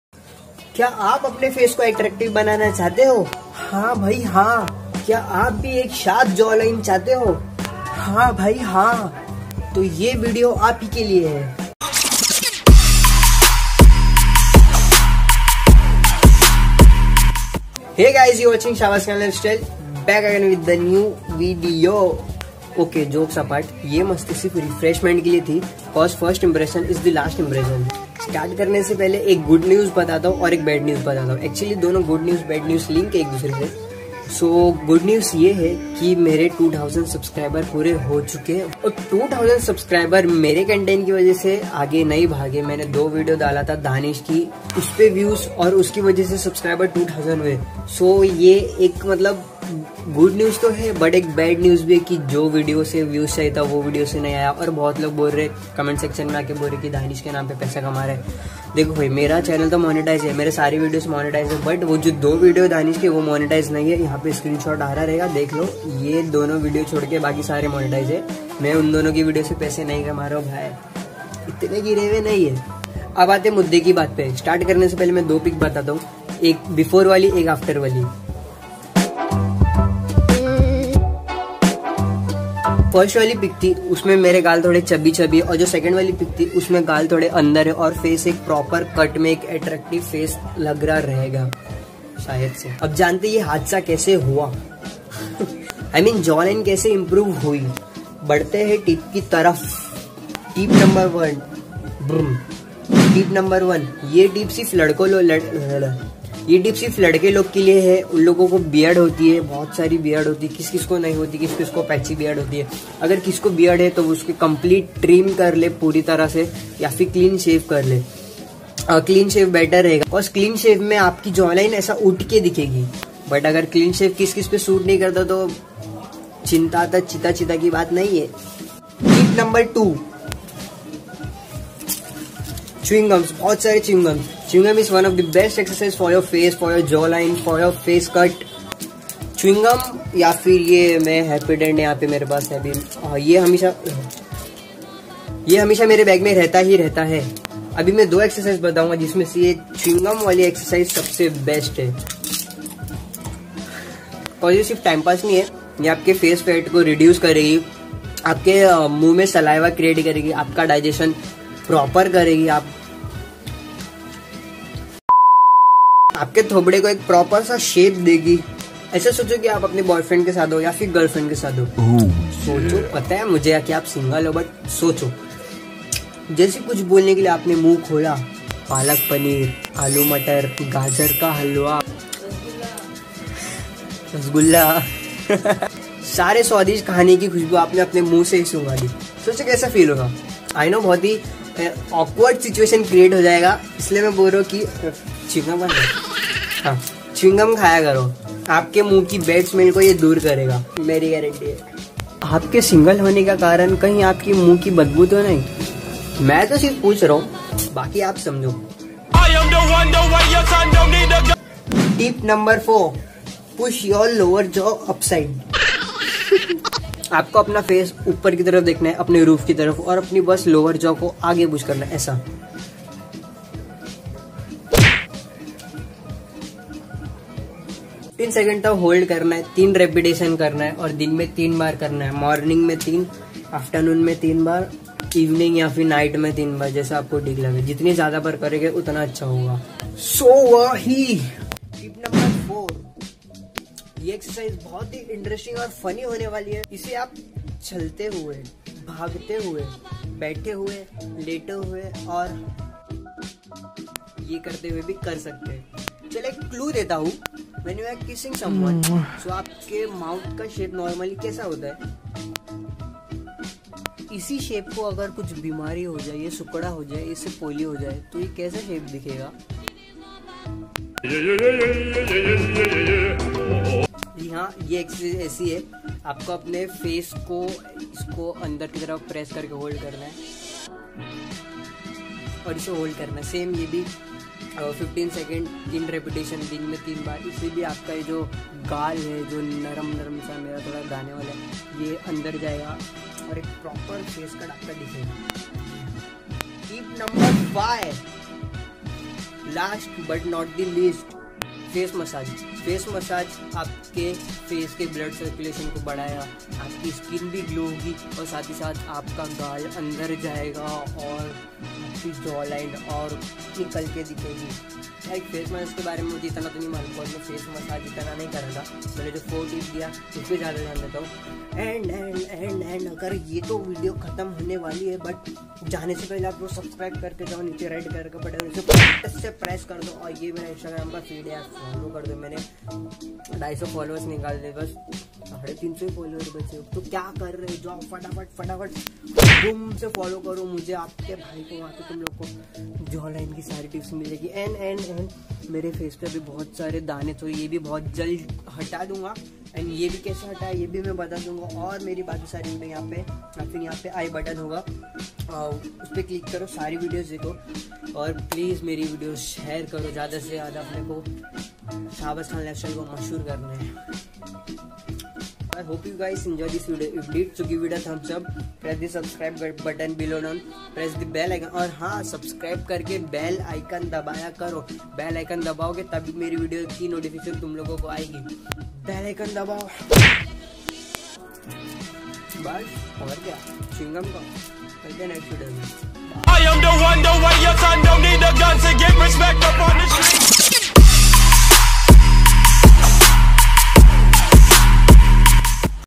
Get sharp jawline within a month | with100% proof
HEY SSUP FAM

I M NOT SHARING THE INFORMATION WHAT I HEARD.
BUT I PERSONALLY TRIED ALL THE EXERCISE & IT'S REALLY WORKS, YOU CAN SEE THE RESULTS WITHIN A WEEK & IT TAKES A MONTH TO GET A PROPER SHARP JAWLINE